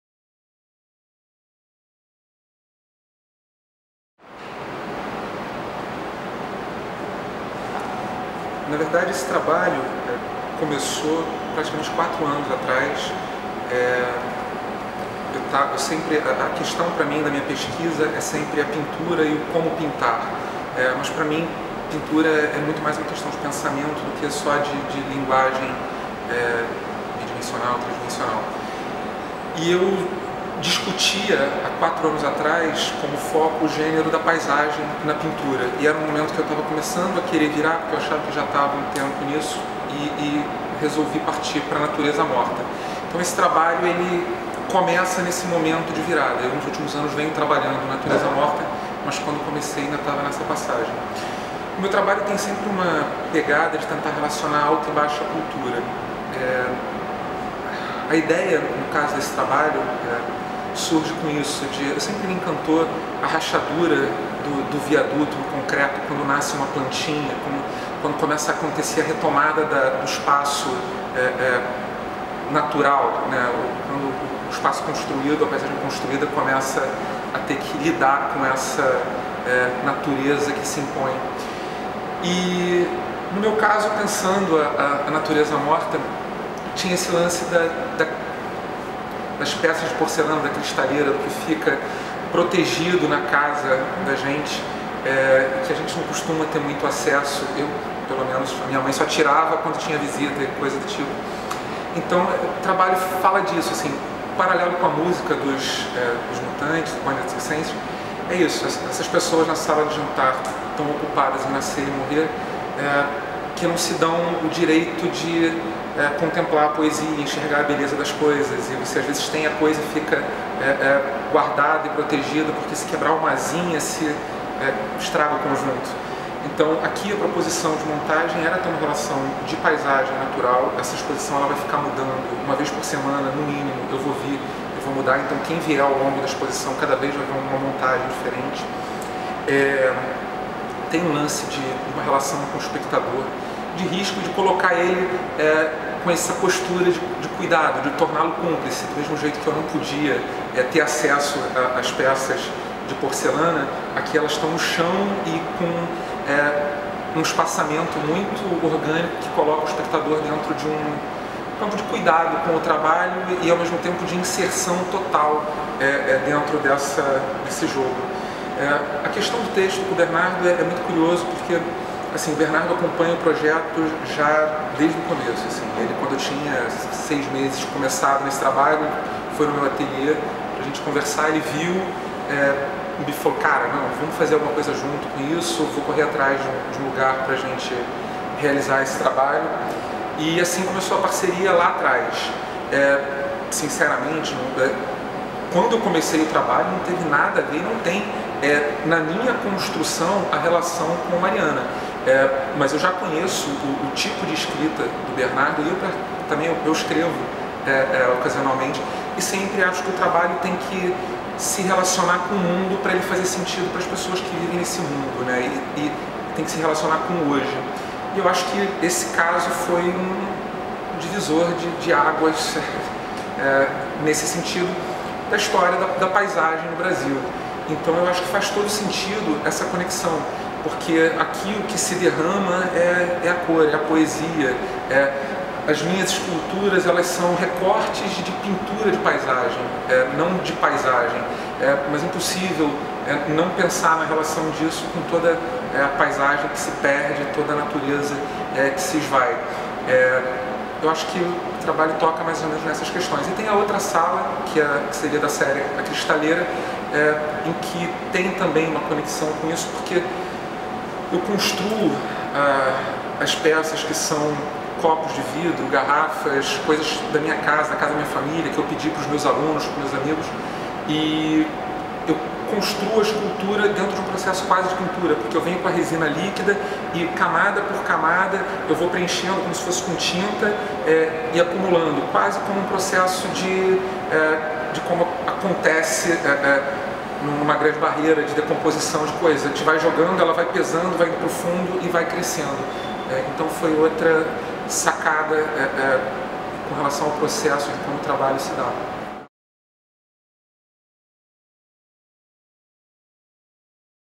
Essas Pessoas da Sala de Jantar - Bruno Miguel [HD]
- Bruno Miguel apresenta duas instalações inéditas no Paço Imperial: "Essas pessoas na sala de jantar" e "Cristaleira". "Essas pessoas..." é formada por 400 composições distintas com peças de porcelana de diferentes origens, espuma de poliuretano, papel maché, pequenas árvores esculpidas pelo artista e tinta de cores vibrantes. Durante o período em que a exposição estará em cartaz, os objetos serão dispostos de forma diferente no espaço, criando percursos distintos pela sala. "Cristaleira" é composta por 150 esculturas dispostas sobre uma prateleira de vidro que percorre as paredes do espaço expositivo. As esculturas são produzidas a partir de objetos utilitários de vidro e cristal, preenchidos com resina de poliéster colorida com spray.